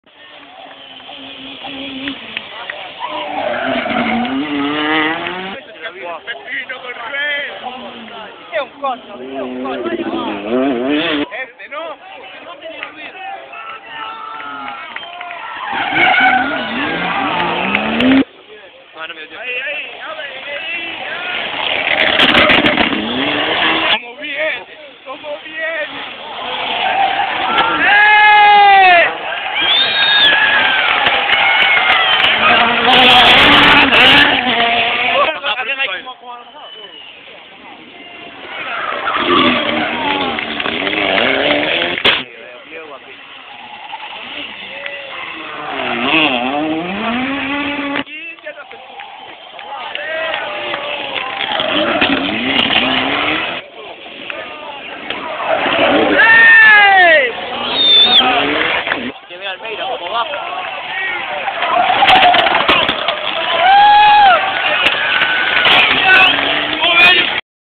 Rally Rias Baixas
Entrada en curva a izkierdas pasadas aseguradas 48 certamen rias baixas!!!